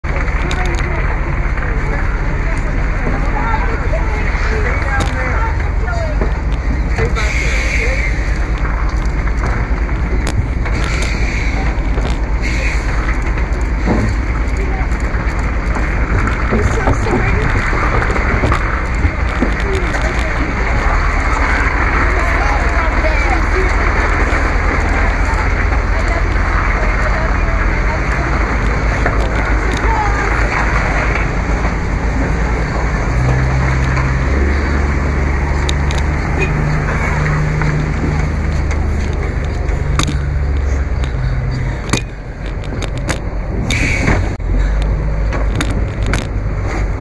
Cows fall in trucks on route to slaughter
What happens if they fall? There is no room to get up.
Cows are forced to lay in manure under bodies of other cows if and when they fall on route to slaughter.